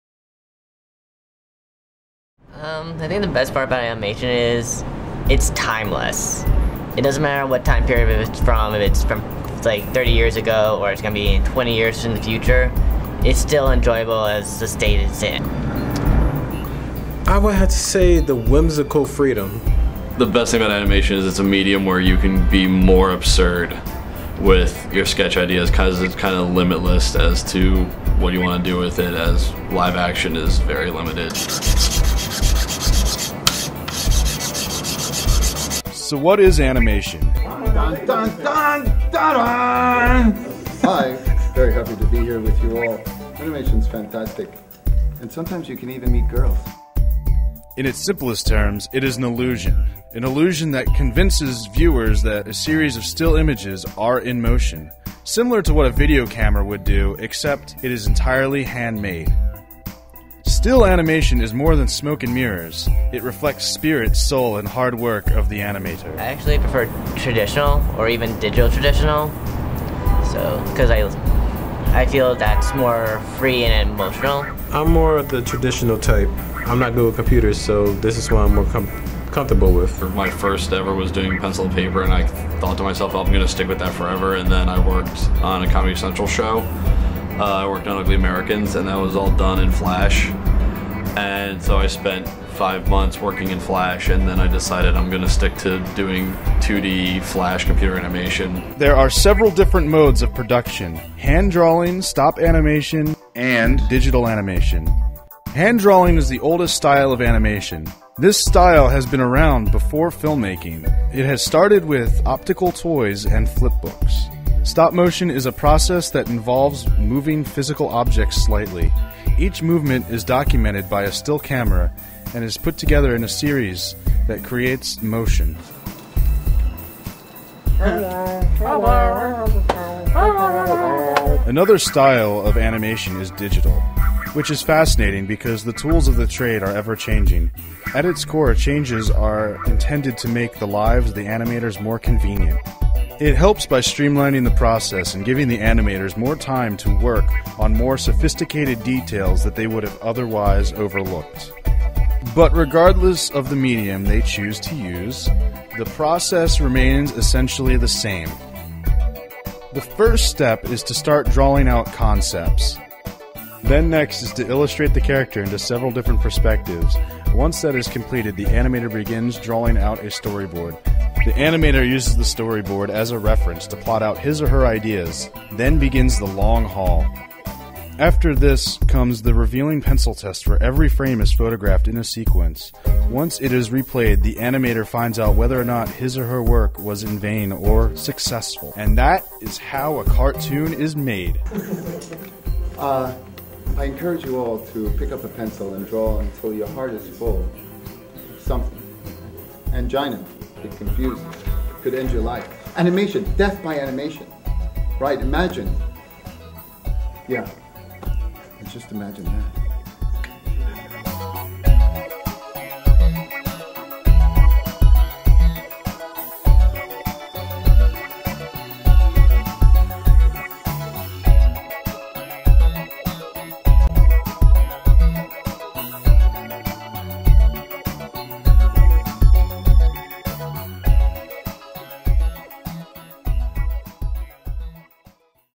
The Art of Animation
A short documentary on animation we worked on for our video production class at University of The Arts. This short features student animators along with the well established animator John Dilworth. He is the creator of "Courage The Cowardly Dog." Shot entirely with a Canon Vixia hf m40.